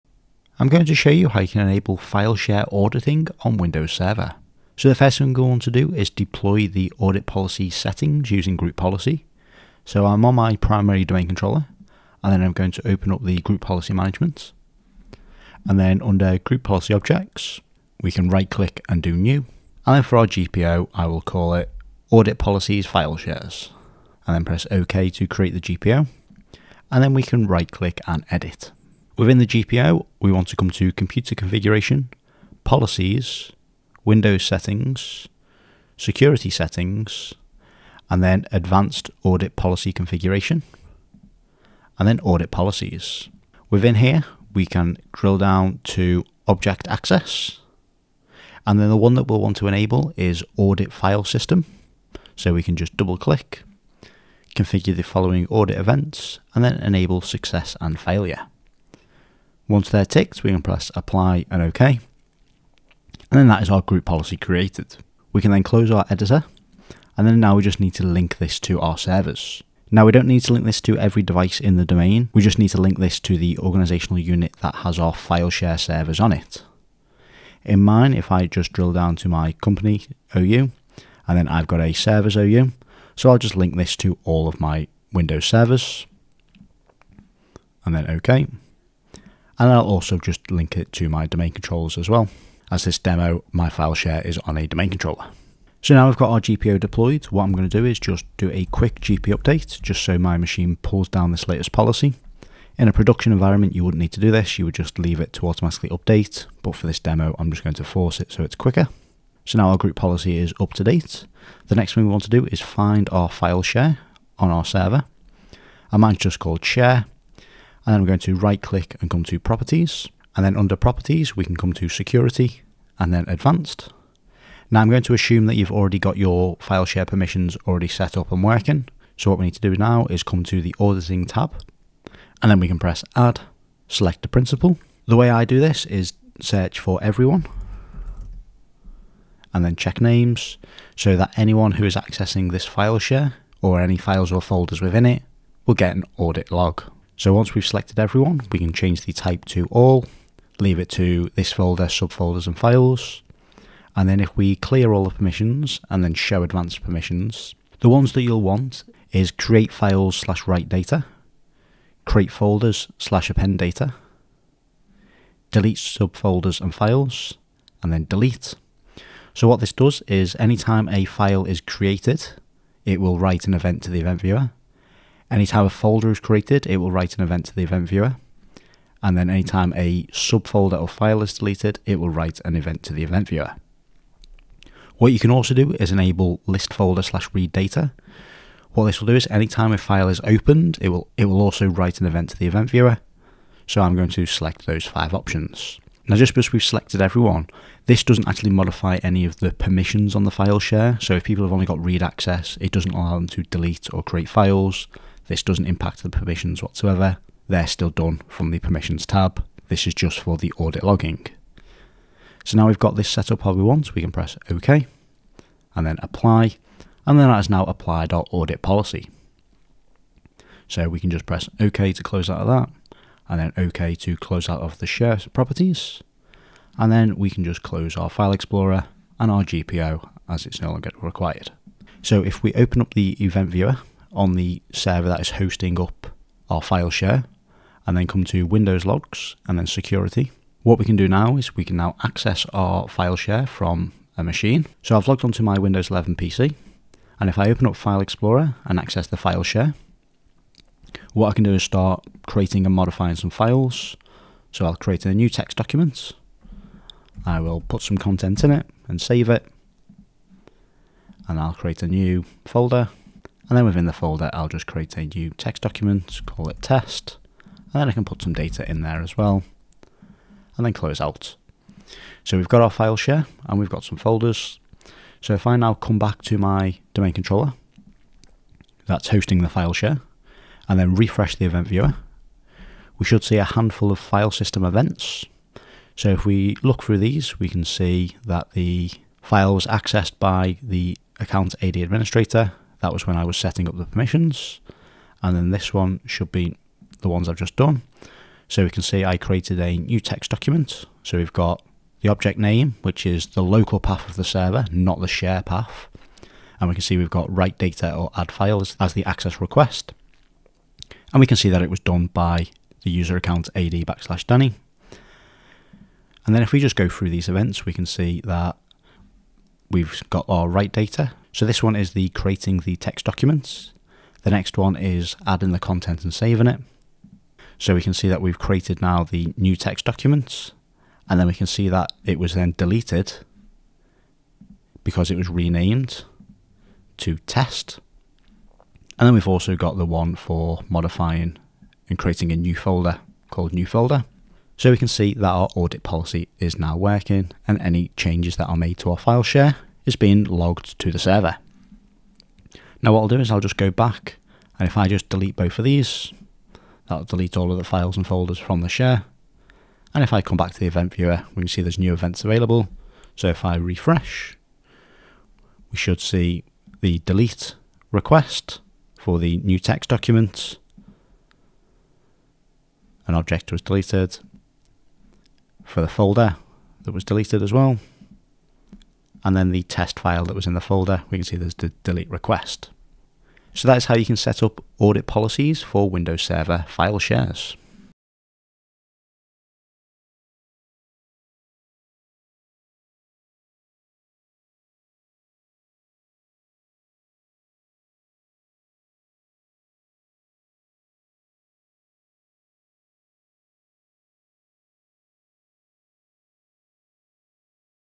How to enable File Share Auditing on Windows Server
Learn how to enable File Share auditing on Windows Server. In this example, I show you how to use Group Policy to deploy Audit Policies to servers and then how to modify the share audit policy so that events are logged to the event viewer.